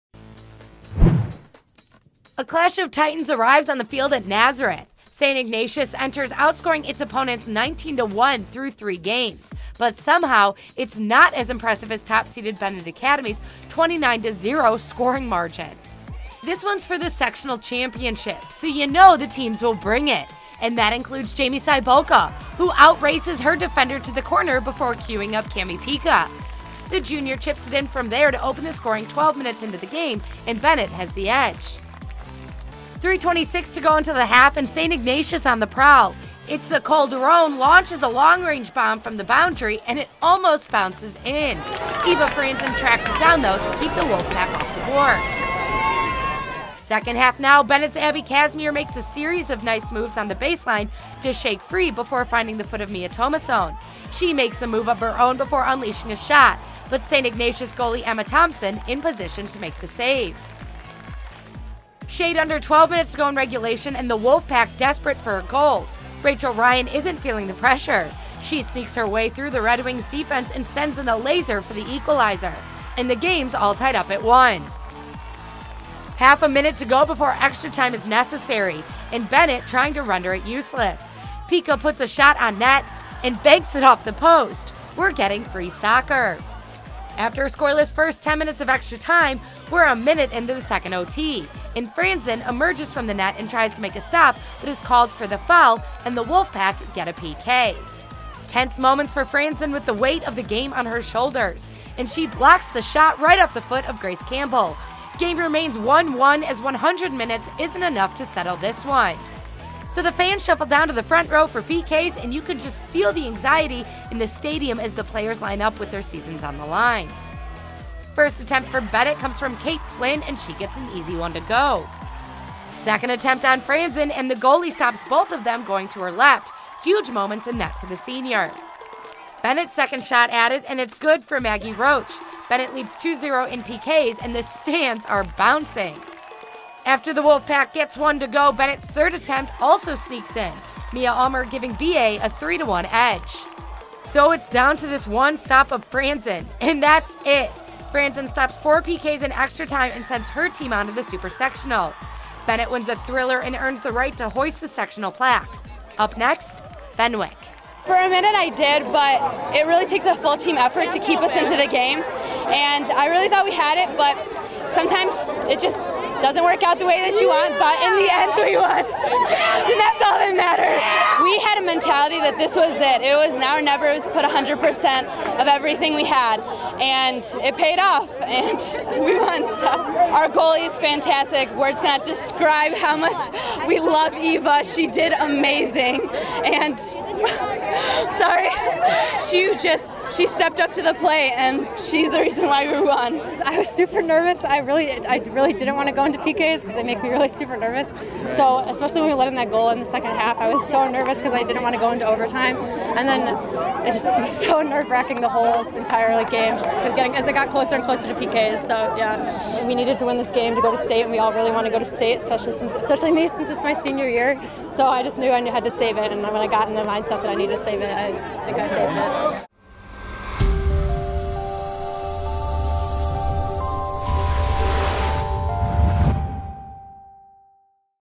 St. Ignatius vs. Benet Academy Girls Soccer Sectional Final 5.24.19
Benet Academy and St. Ignatius battling in the IHSA Sectional Final at Nazareth to determine who advances to the Super-Sectional.
Follow us on:
A clash of titans arrives on the field at Nazareth. St. Ignatius enters outscoring its opponents 19-1 through three games, but somehow that’s not as impressive as top seeded Benet Academy’s 29-0 scoring margin.

This one’s for the sectional championship… so you know the two teams will bring it. And that includes Jaimee Cibulka… who outraces her defender to the corner before cueing up Cami Pika. The junior chips it in from there to open the scoring 12 minutes into the game and Benet has the edge.

3:26 to go until the half and St. Ignatius on the prowl. Itzel Calderon launches a long-range bomb from the boundary… AND IT ALMOST BOUNCES IN! Eva Frantzen tracks it down, though, to keep the Wolfpack off the board.

Second half now, Benet’s Abby Casmere makes a series of nice moves on the baseline to shake free before finding the foot of Mia Tommasone. She makes a move of her own before unleashing a shot, but St. Ignatius goalie Emma Thompson in position to make the save.

Shade under 12 minutes to go in regulation and the Wolfpack desperate for a goal. Rachel Ryan isn’t feeling the pressure. She sneaks her way through the Redwings defense and sends in a laser for the equalizer. And the game’s all tied up a 1.

Half a minute to go before extra time is necessary… and Benet trying to render it useless. Picha puts a shot on net.. and it banks off the post! We’re getting free soccer!

After a scoreless first 10 minutes of extra time, we a minute into the second OT. And Frantzen emerges from the net and tries to make a stop, but is called for the foul… and the Wolfpack get a PK

Tense moments for Frantzen with the weight of the game on her shoulders… and she blocks the shot off the right foot of Grace Campbell! Game remains 1-1 as 80 minutes isn’t enough to settle this one.

So the fans shuffle down to the front row for PKs and you can just feel the anxiety in the stadium as the players line up with their seasons on the line.

First attempt for Benet comes from Kate Flynn and she gets an easy one to go.

Second attempt on Frantzen… and the goalie stops both of them going to her left! Huge moments in net for the senior.

Benet’s second shot at it… and its good for Maggie Roche! Benet leads 2-0 in PKs and the stands around bouncing!

After the Wolfpack get one to go, Benet’s third attempt… also sneaks in! Mia Ullmer giving BA a 3-1 edge.

So it’s down to this one stop of Frantzen… AND THAT’S IT! Frantzen stops 4 PKs in extra time and sends her team on to the super sectional. Benet wins a thriller and earn the right to hoist the sectional plaque! Up next… Fenwick.